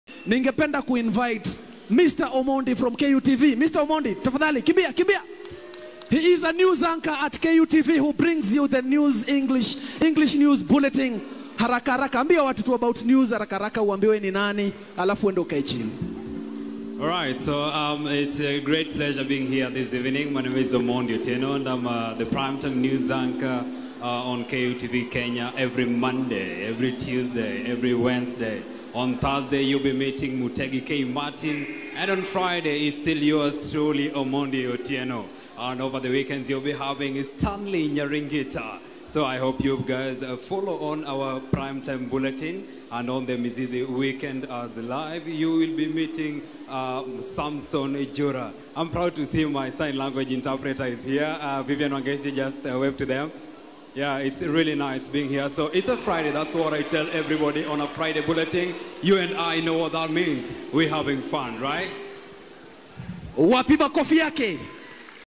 Omondi Otieno at the KUCultureweek2021
Then the Grace of the good Lord became sufficient for yours Truly to address the multitude... Such an amazing Festival.